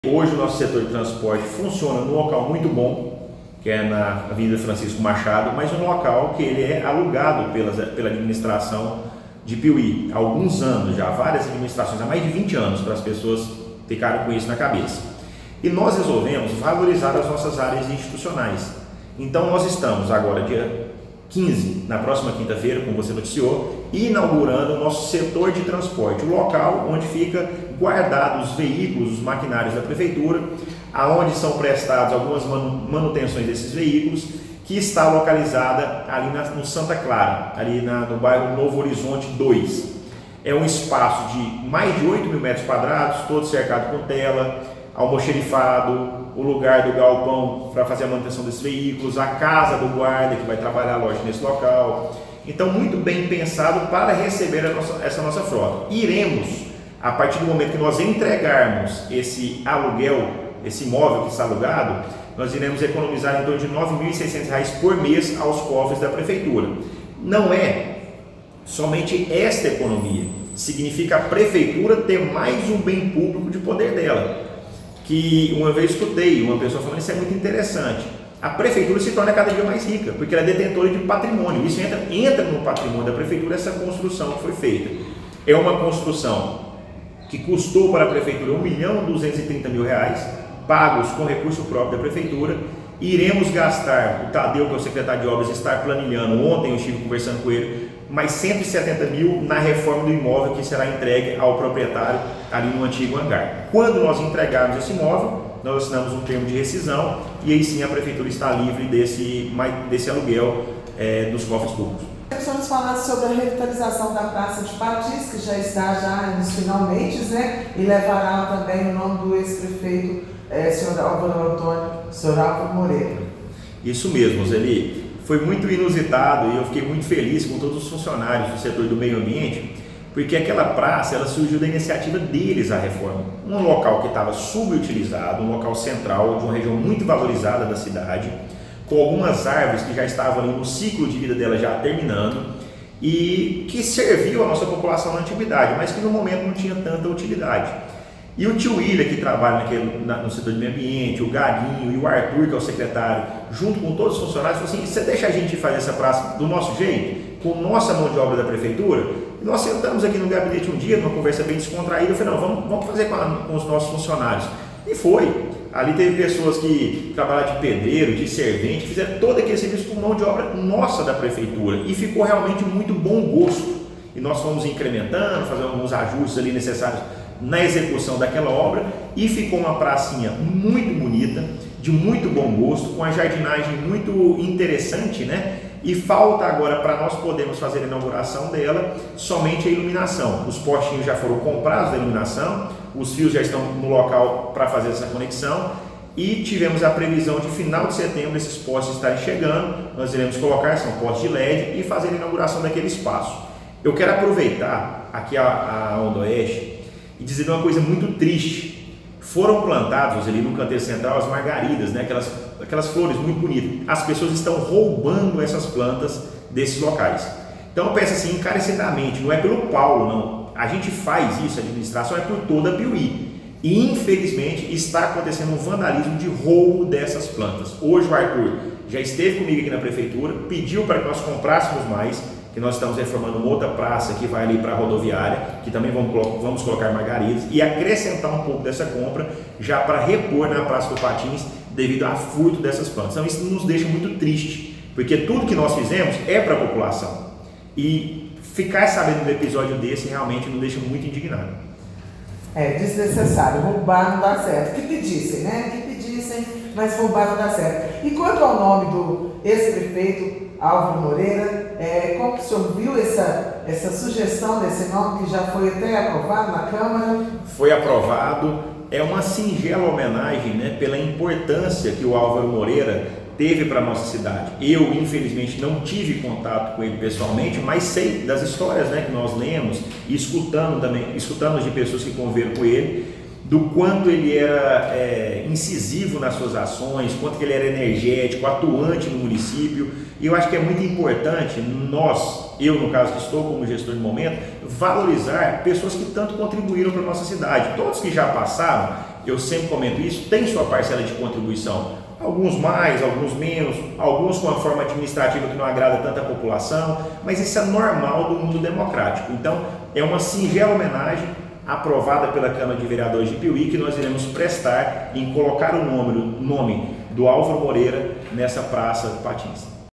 PIUMHI ECONOMIZARÁ MAIS DE 9 MIL REAIS POR MÊS COM NOVA SEDE DO HANGAR
| PIUMHI |